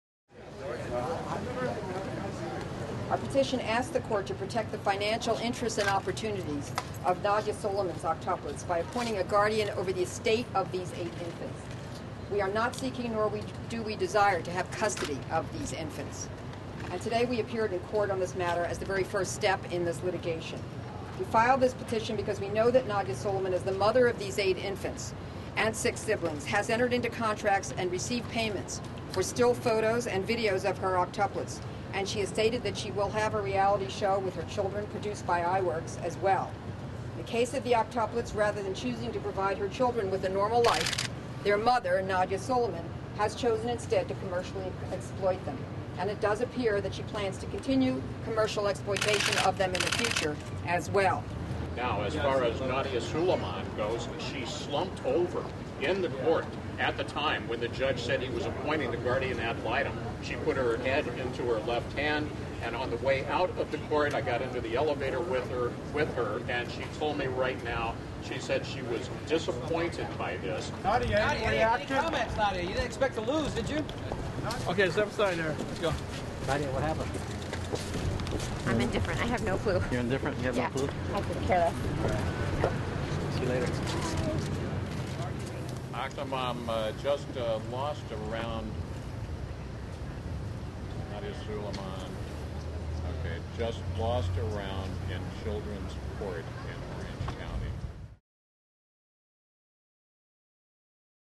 Octuplets' mom loses court decision: "I could care less." - 2009-07-28
Nadya Suleman, the mother of octuplets, has a guardian for her children appointed by an Orange County court. Video by Eugene Garcia, ocregister.com Published on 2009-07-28